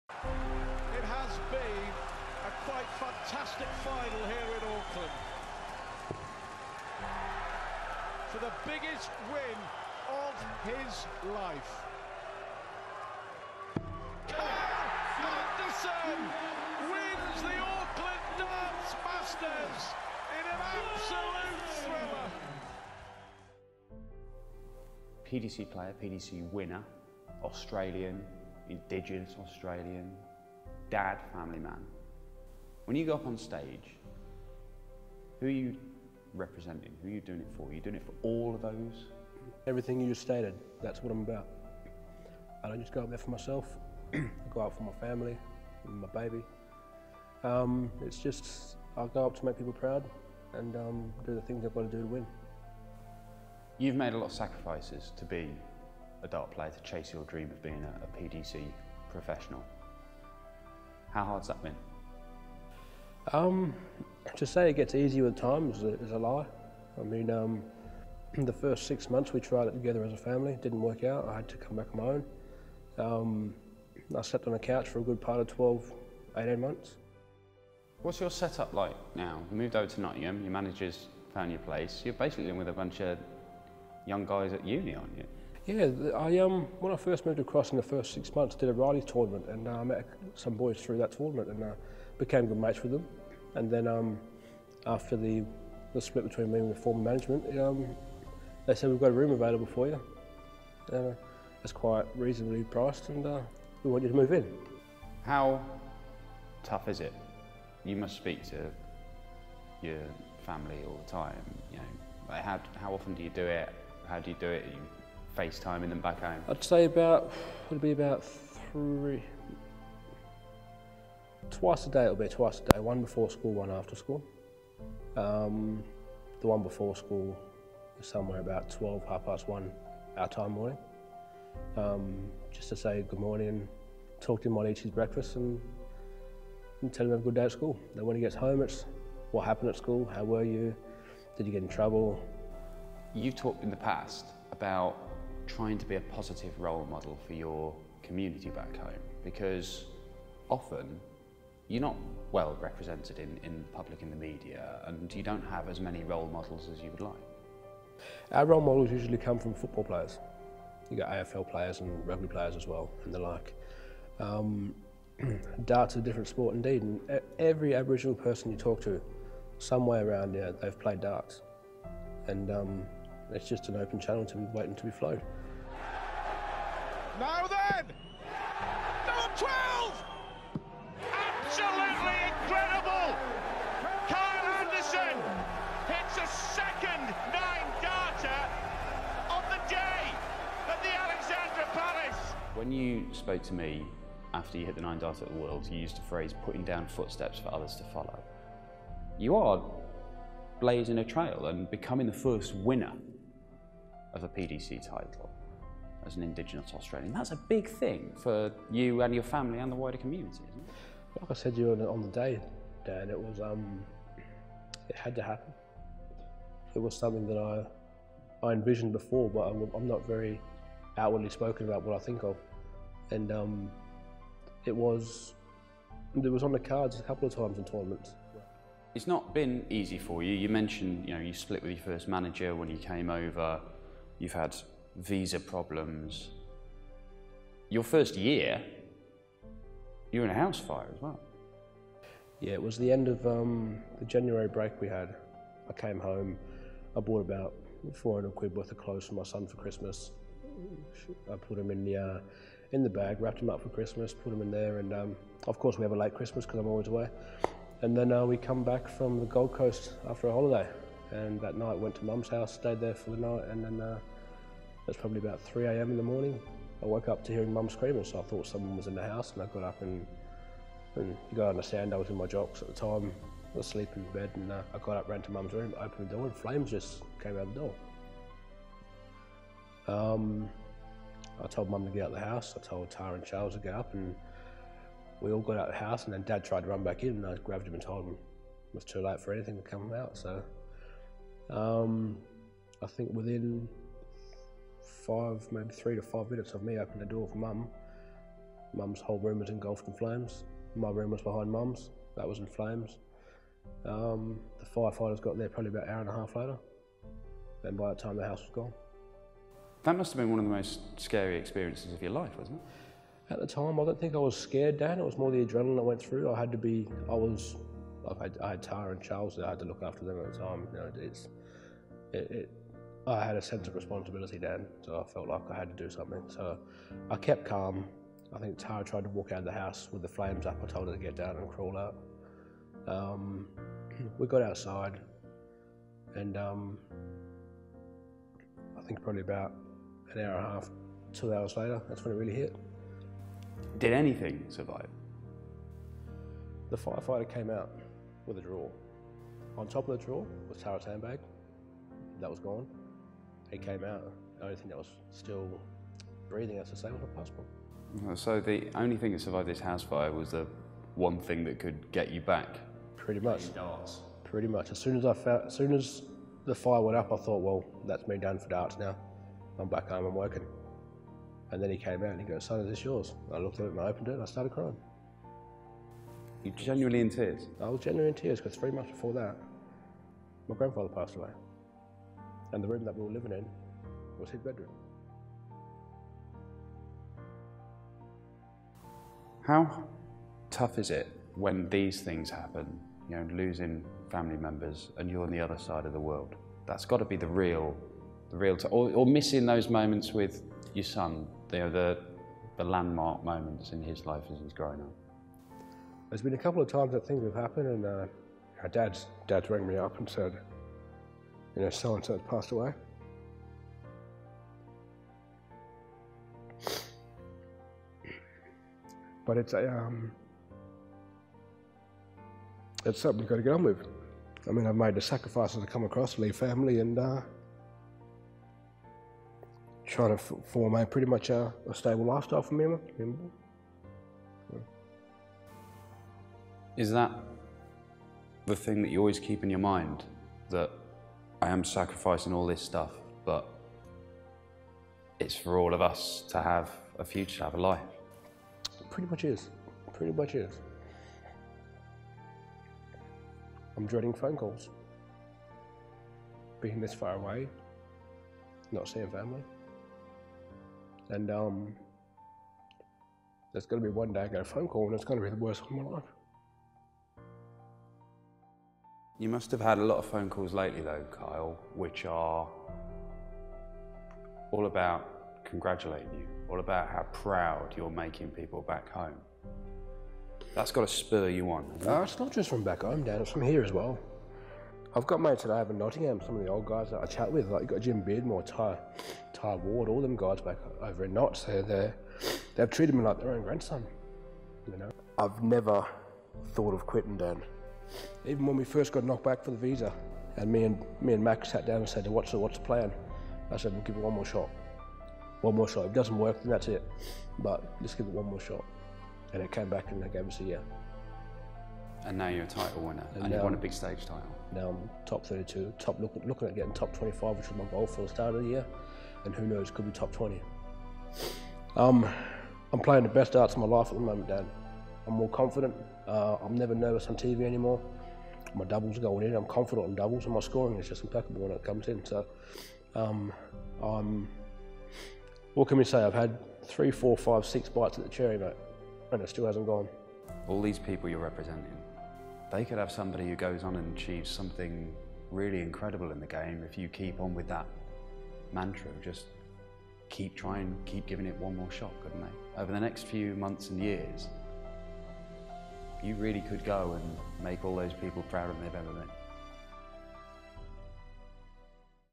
MEET THE PLAYER! An emotional Kyle Anderson reflects on the sacrifices he has made
Kyle Anderson reflects on leaving his family and surviving a house fire in his pursuit of success on the Oche.